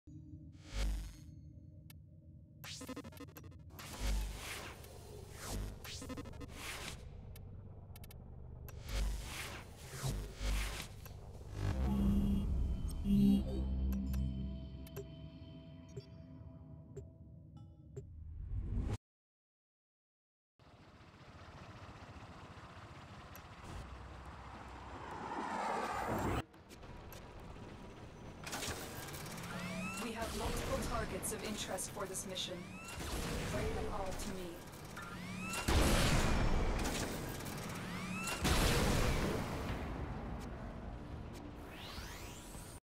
Ogris no bueno
No explosion visual or explosion sound effect. Bug originally reported nearly a month ago

Edit: Fixed, finally! :D